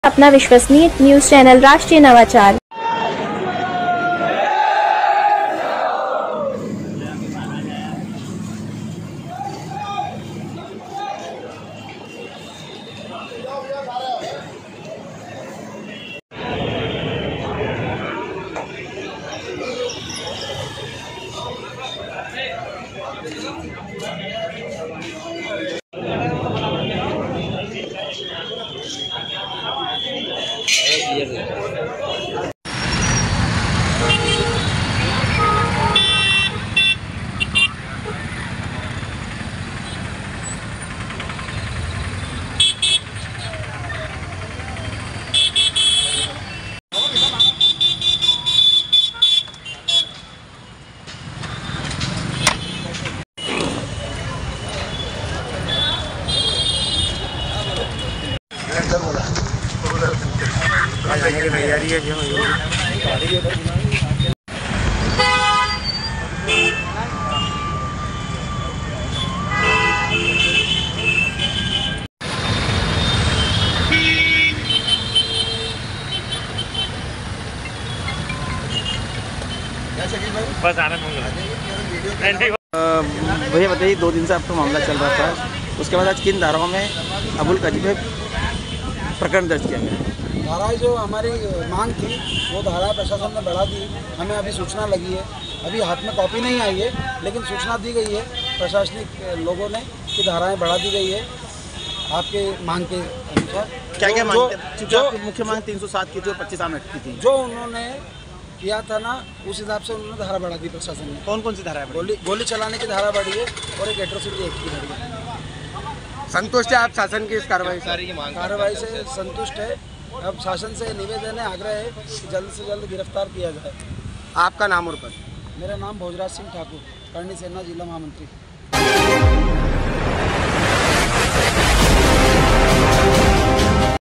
शहरकाजी फारूखी पर 307 व आर्म्स एक्ट। विक्रमसिंह पवार हिंदू संगठन के कार्यकर्ताओं से बात करने पहुंचे।
हिंदू संगठन के कार्यकर्ताओं से बात करने पहुंचे विधायक पुत्र विक्रमसिंह पवार। कोर्ट में फरियादी के 164 में हुए बयान के आधार पर विवेचना में शहर काजी अबुल कलाम फारूखी पर धारा 307 व आर्म्स एक्ट, अट्रोसिटी एक्ट की धारा बढ़ाई गई। मांग पूरी हुई अब जल्द से जल्द गिरफ्तारी हो, यही अपेक्षा की कार्यकर्ताओं ने।